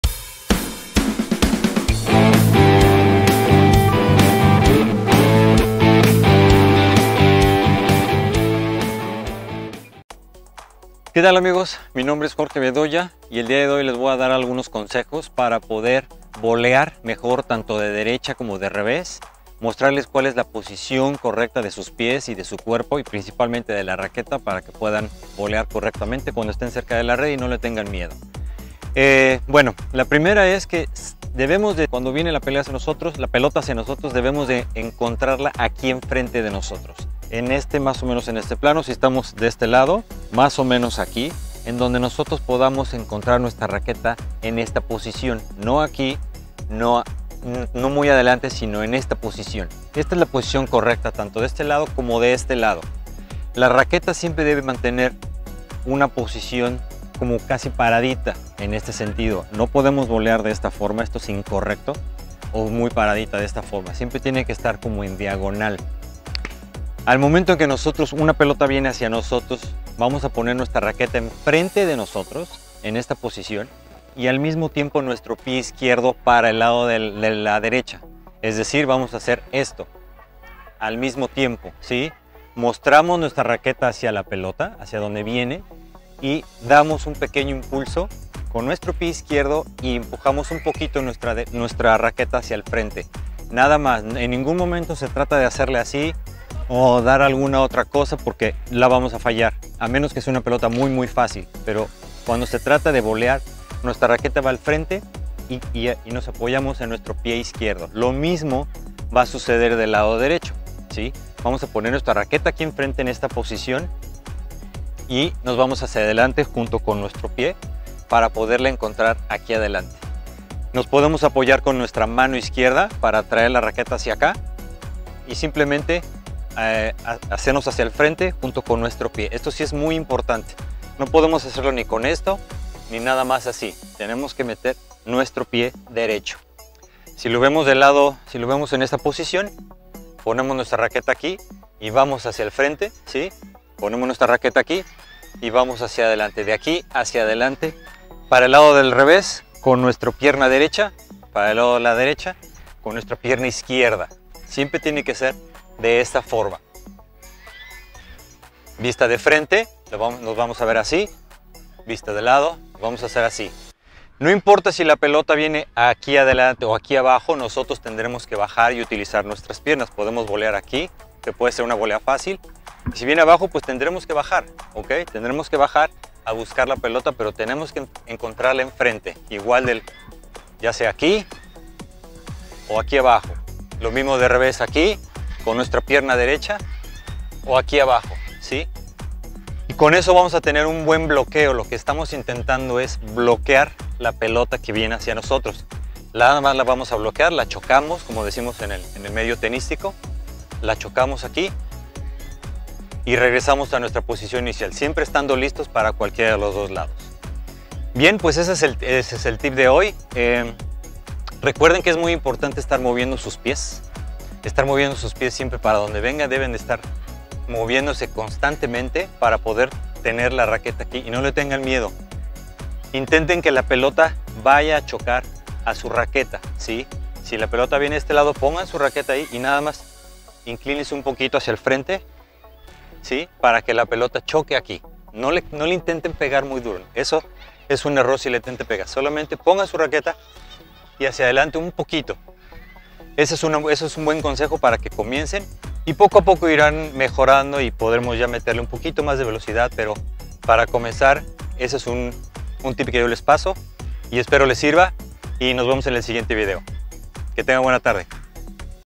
VOLEA de Tenis para principiantes, sencillos tips!
Aprende algunos tipos de tenis para mejorar tu volea y no tener miedo al subirte a la red. Con estos sencillos tips podrás y una buena posición podrás volear sin miedo.

(Aplica restricciones. Solo para USA)

El juego de red en el Tenis es importante cuando estamos cerca de la red y ganar puntos o bien cuando jugamos dobles de Tenis Tennis. Les mostarpe como volear de forma sencilla en ambos lados derecha como revés haciendo un movimiento corto pero efectivo.